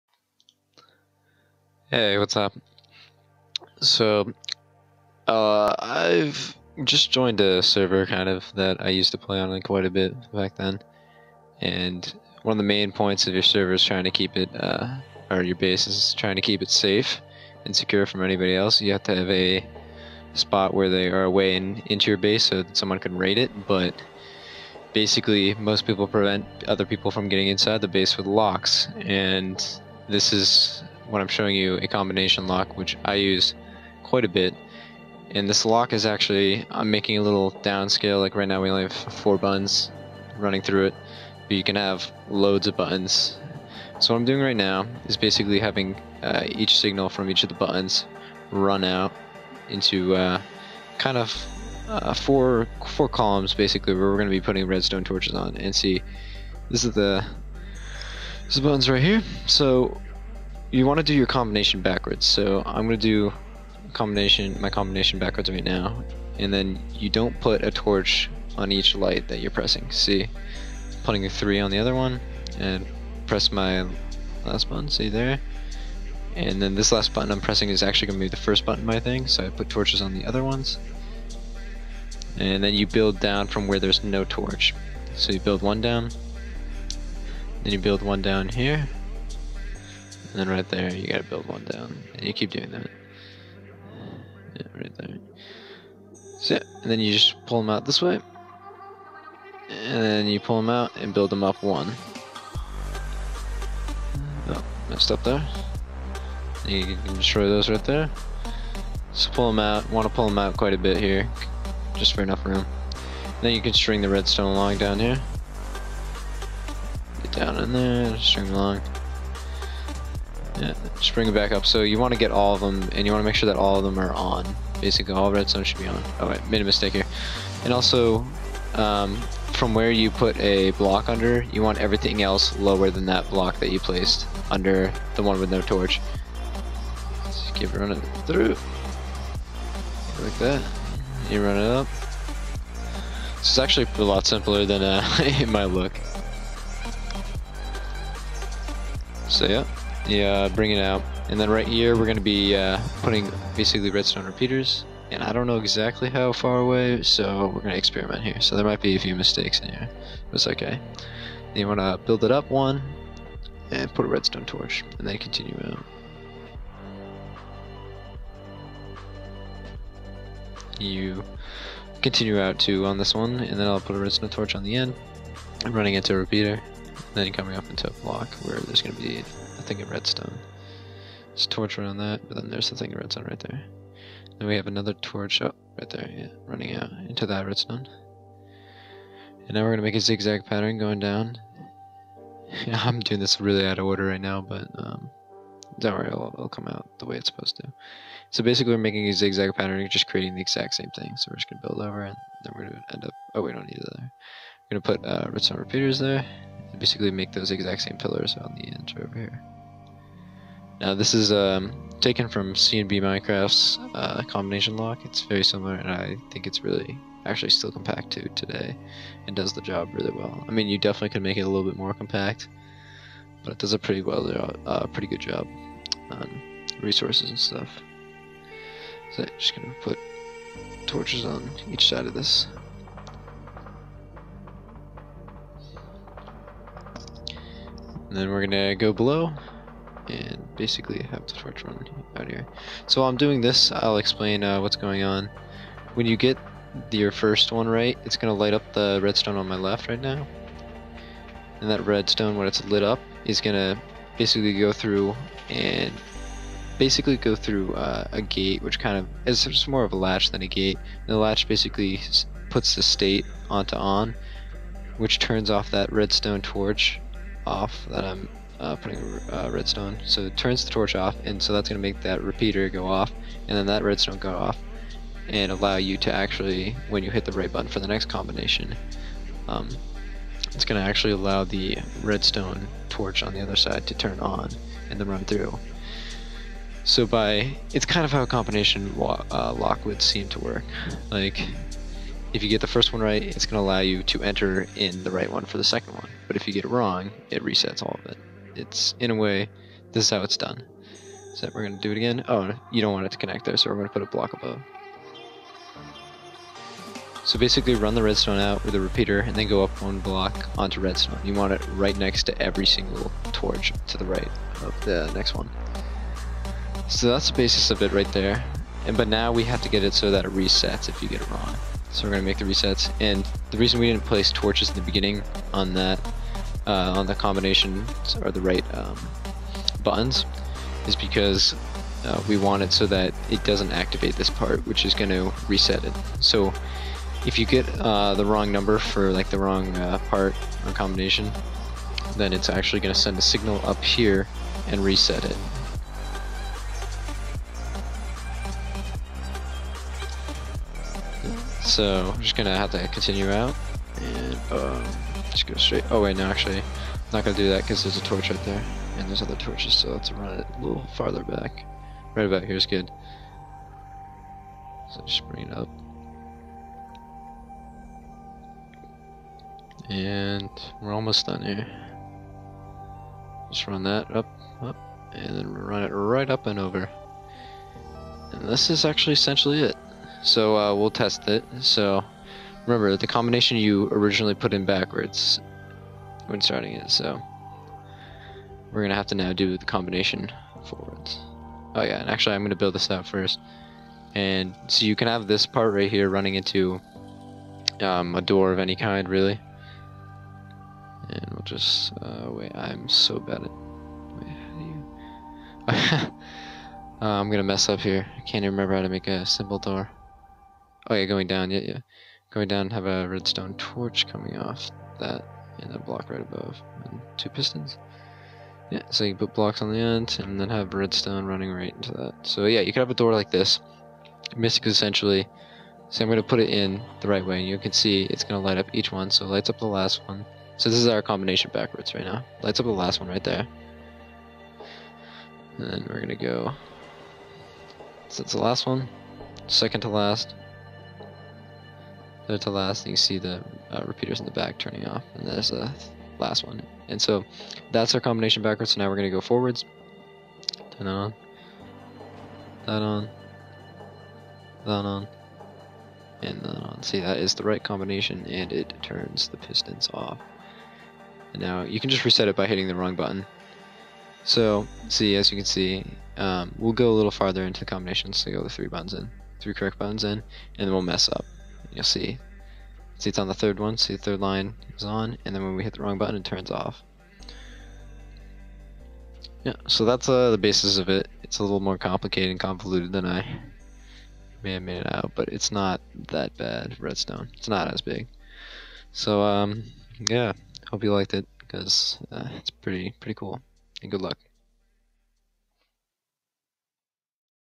Combination Lock #1
Based off and very similar to CNB's combination lock. A small video to explain and show how to build one. It'll help stop people from breaking into your base.
Music